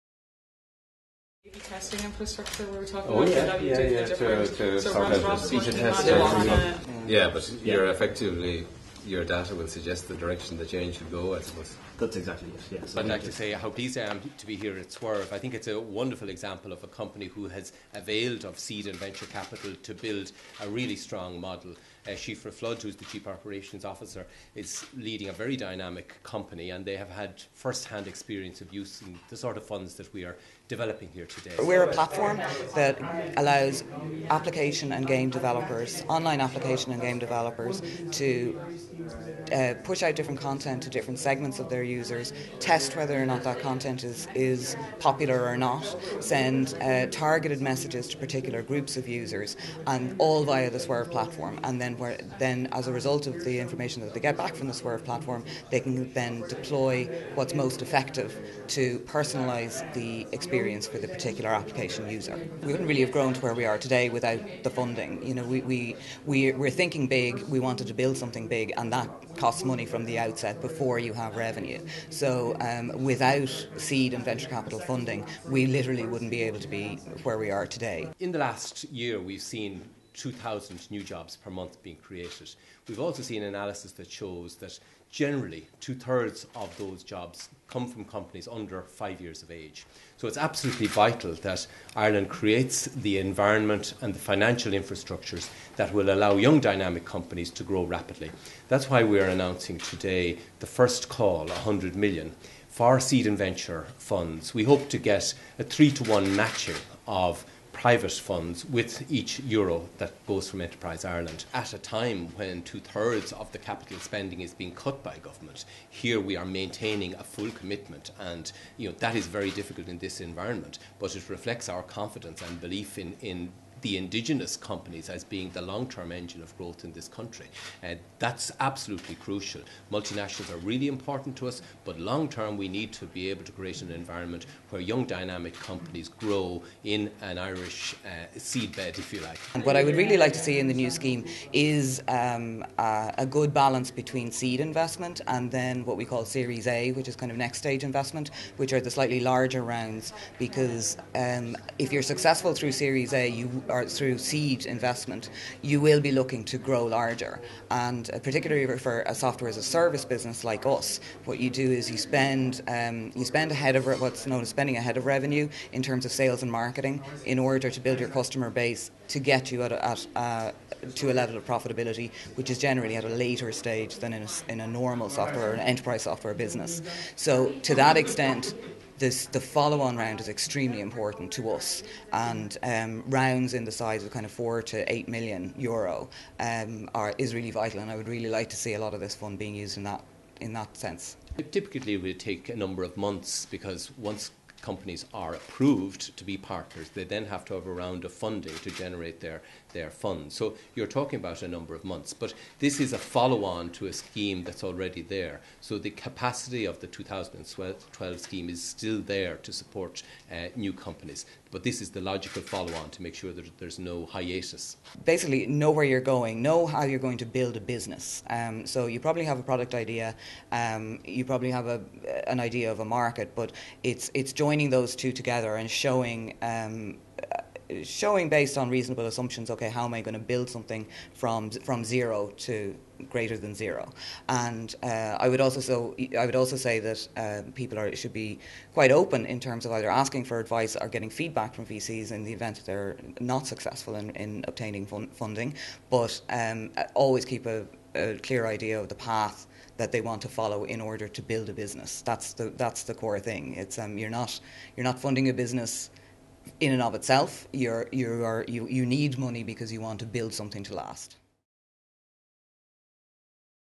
Minister Bruton Announces New €175million Government Seed and Venture Capital Scheme
Jobs Minister Richard Bruton today launched a new €175million Seed and Venture Capital Scheme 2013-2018 aimed at providing additional funding for high-growth Irish companies with the potential to generate large amounts of additional export sales and grow jobs.
The Minister today issued the first call for Expressions of Interest from venture capital funds seeking investment, and advertisements will appear in newspapers from this weekend. Minister Bruton is targeting an additional €525million in funding from the private sector, which will mean a total of €700million available for investment over the lifetime of the Scheme. It is expected that the average initial venture investment in a company made by the funds targeted under this call could be of the order of €2million.
Under the Scheme, the Department of Jobs, Enterprise and Innovation, through Enterprise Ireland, will make funding available for investment in private seed and venture capital funds. These funds, in turn, will invest in high-growth innovative Irish companies in fast-growing sectors including ICT, life sciences, high-tech manufacturing and the green economy.
This first call for expressions of interest will see Enterprise Ireland committing up to €100m to venture capital funds targeting investment in the wider ICT and life sciences sectors. It is expected that over the lifetime of the scheme it will benefit hundreds of Irish companies, generate tens of millions of euros in additional export sales and create thousands of jobs.
Making the announcement, Minister Bruton said:
I am determined to ensure that we support the creation of a powerful engine of Irish enterprise that can sustain and create the levels of employment we need. A key part of the Government's plans to support Irish businesses is to provide access to credit for a range of different types of businesses, from micro-enterprises employing fewer than 10 people up to mid-sized and large engineering companies in need of development capital.
Today I am very happy to launch the new Seed and Venture Capital Scheme which will run over the next six years and make investments in hundreds of high-growth, innovative Irish companies in sectors which we have targeted through the Action Plan for Jobs including high-tech manufacturing and ICT. These are the companies that could develop into the next major success story for the Irish economy. International experience from Silicon Valley and elsewhere shows venture capital can play a key part in ensuring access to mentoring and networks as well as the funding that supports the growth of these companies.
Frank Ryan, Chief Executive, Enterprise Ireland, commented:
The world of Venture Capital investing is changing and we are particularly interested in receiving Expressions of Interest that reflect current thinking in the market. Proposals are sought that will deliver 'Smart Capital' to investees as the provision of advice, support, and access to networks in addition to any capital invested is vital to the development of our companies to ensure that they can continue to grow and create jobs in Ireland.